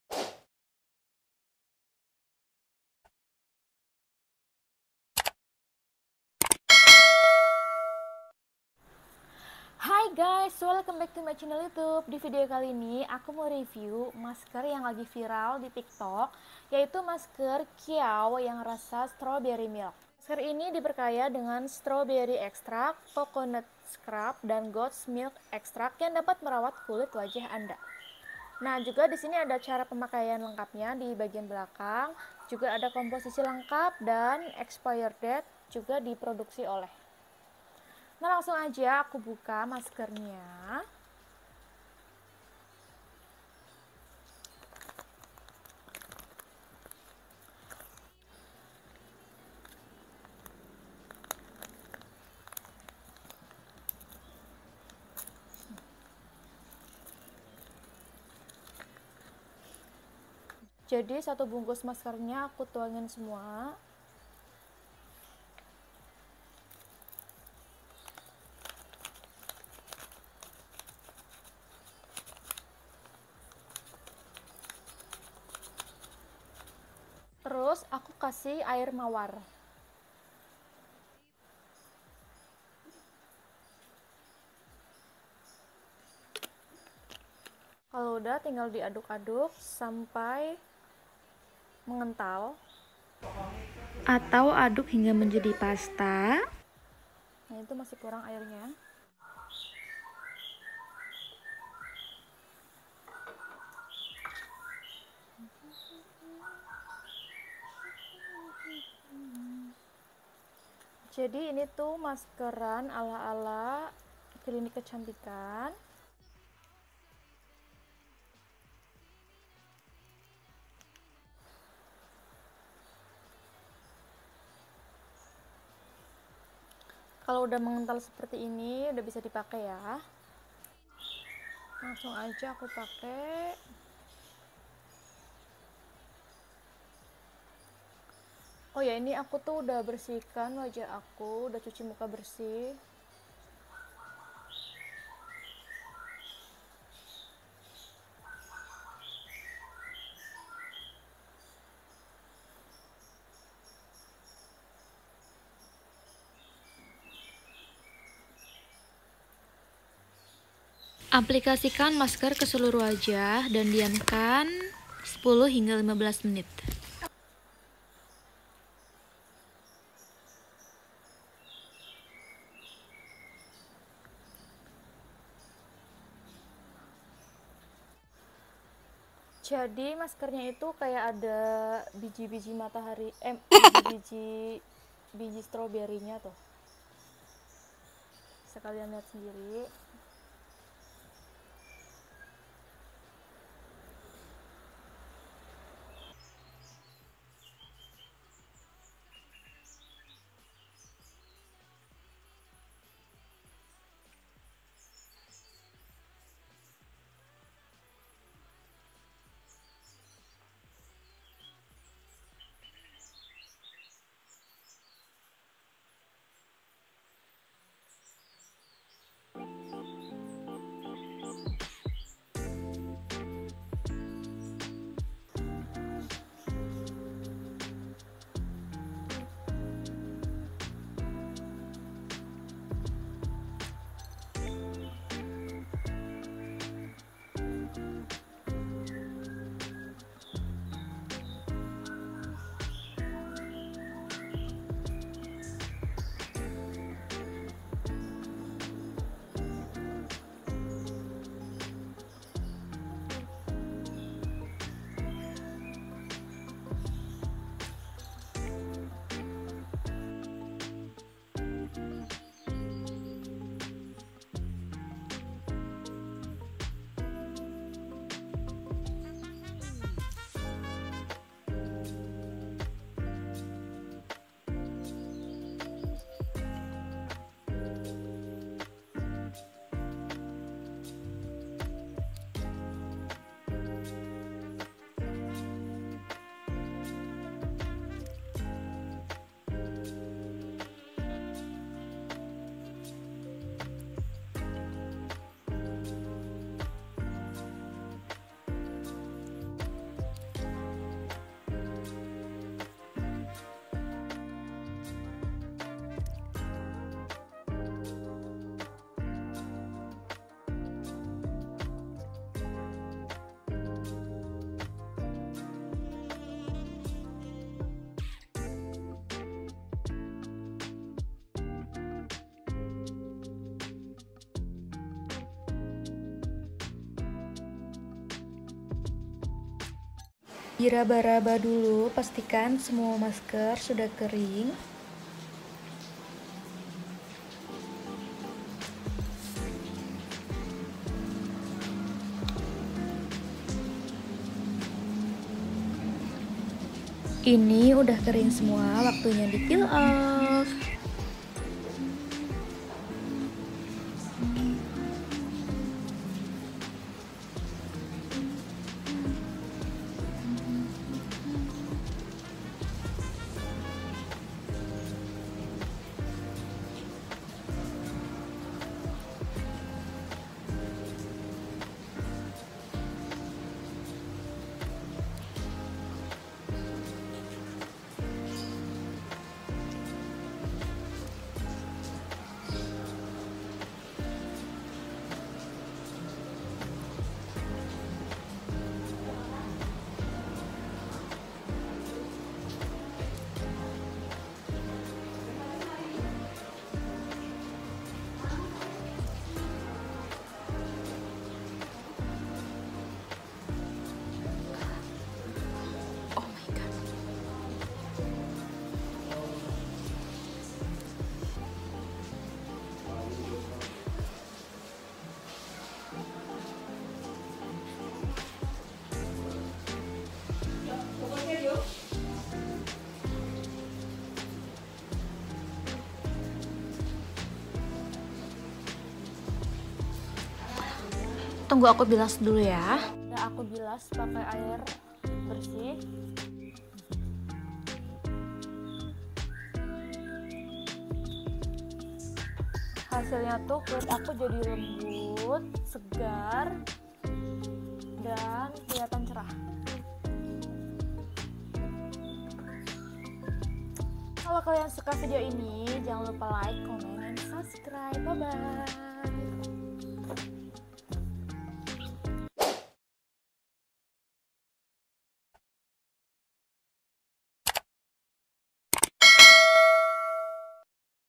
REVIEW PEEL OFF MASK BY KYAU / MASKERAN DI RUMAH ALA SALON KECANTIKAN
Hai guyss Terimakasih sudah tonton video terbaru ku…

Di video kali ini aku mau maskeran di rumah ala salon kecantikan pakai masker Kyou yang varian strawberry milk… bau masker nya enak bangettttt bau bau susu strawberry gitu hehe… dan setelah pakai masker nya muka aku jadi seger bangettt , halus dan lebih cerah lohh..

Nah biar gak penasaran tonton yaa video nya ….

Aku beli masker nya di gallery Skincare Gombong harga nya Rp 8,000